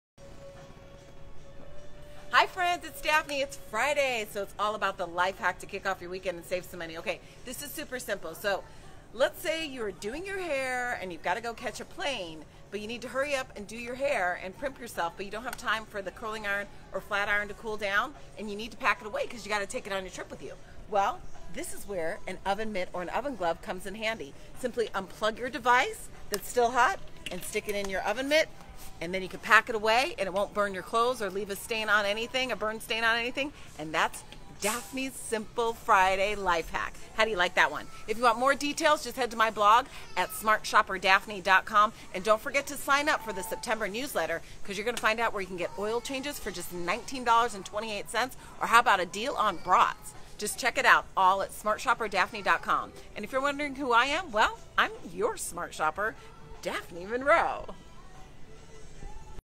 Daphne’s Friday Life Hack oven mitt to cool curling iron
Oven mitten or gloves can come handy when you need to curl your hair before catching a flight and you're short on time.